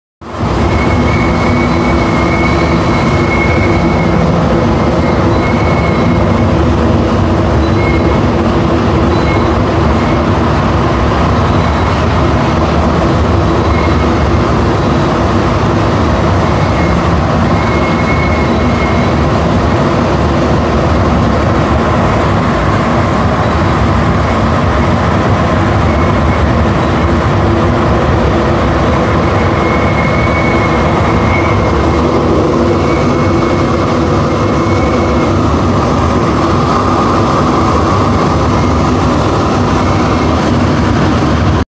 Is AZUR Set 36 haunted by ghosts? Listen to this sound
Just having a little fun haha. I assume this sound came from the vents.

I often hear this whistle sound on AZUR trains.